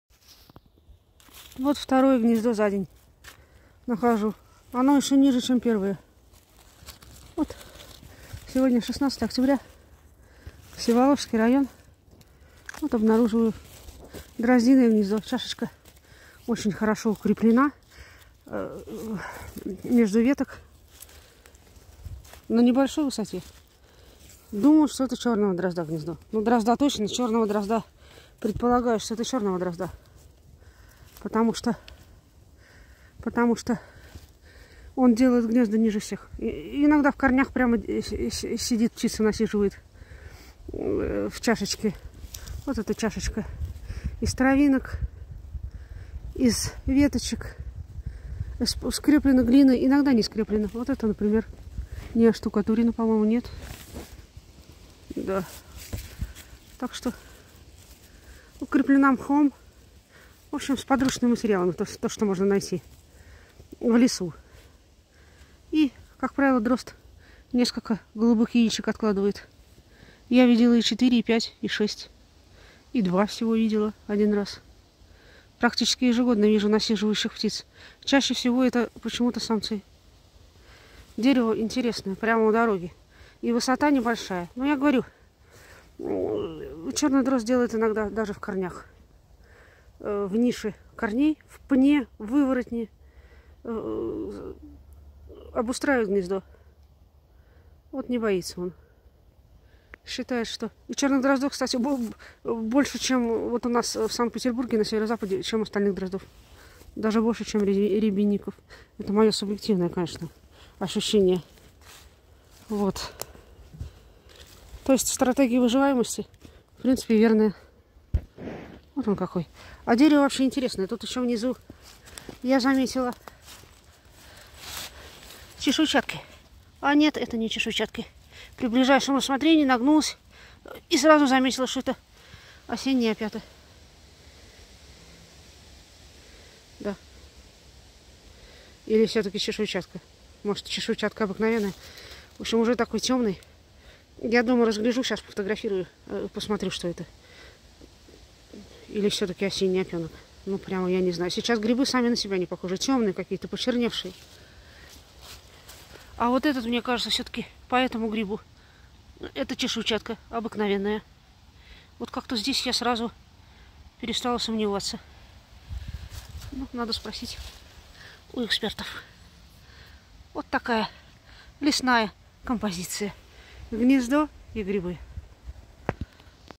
Неожиданная находка на дереве
Обнаружила в развилке толстых веток гнездо дрозда. Что дрозд - это 100%. А какой? Полагаю, что чёрный. На такой небольшой высоте, в метре от земли, чаще всего строит гнездо именно чёрный.
У корней дерева обнаружила гриб - чешуйчатку обыкновенную. Гриб не слишком съедобен, хотя некоторые грибники его солят или маринуют вместе с опятами и другии чешуйчатками. И называют "королевским опёнком".
Но гриб невкусный. В некоторых источниках упоминается, что могут быть лёгкие отравления. В общем, вопрос дискуссионный)